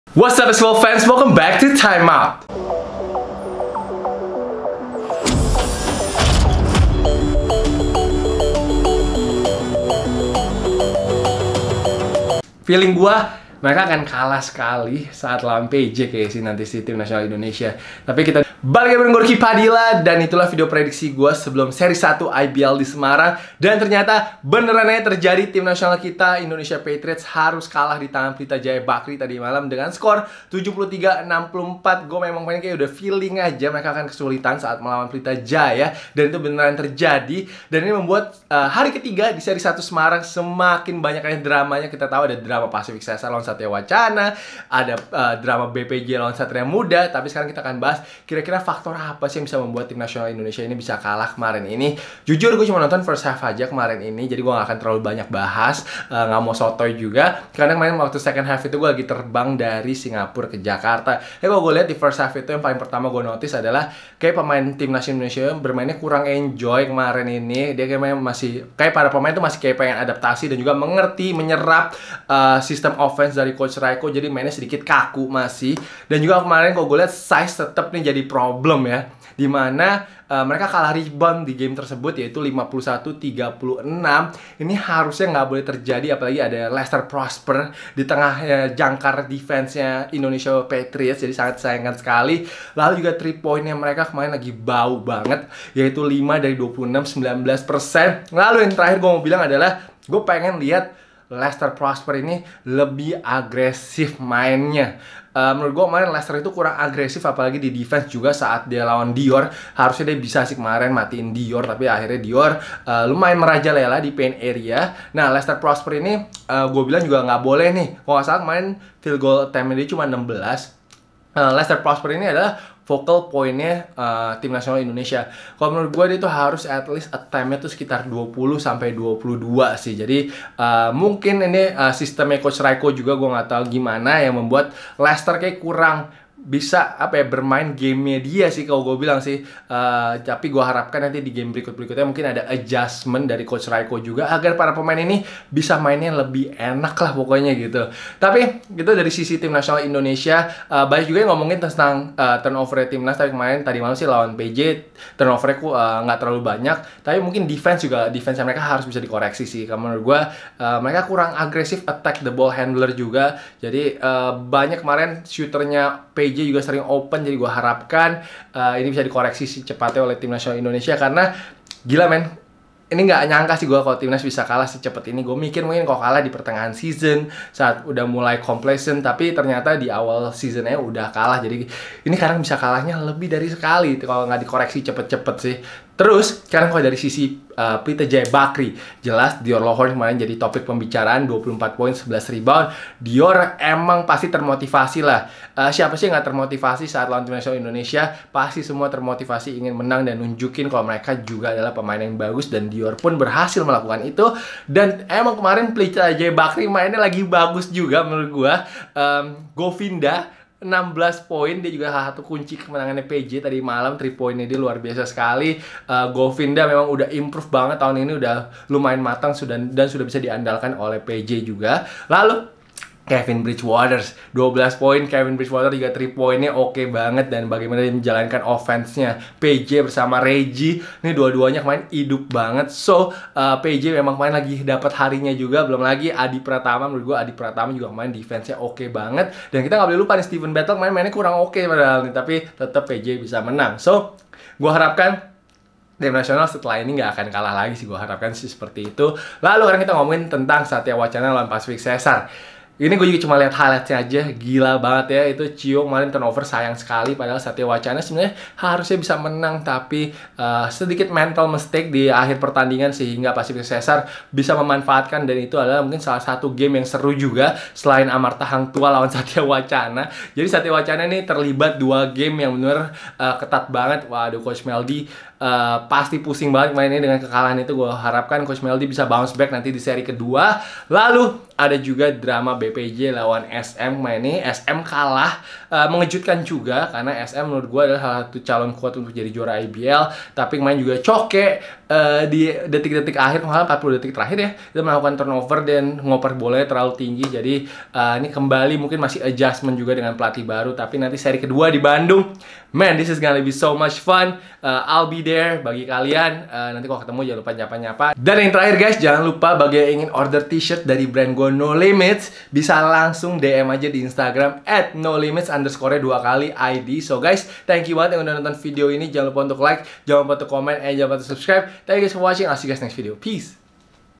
Time Out #342: Apa Yang Salah Dari Kekalahan Tim Indonesia Patriots?
Mengulas kekalahan pertama Indonesia Patriots di seri 1 IBL Semarang.

Thumbnail photo via @PelitaJayaBasketball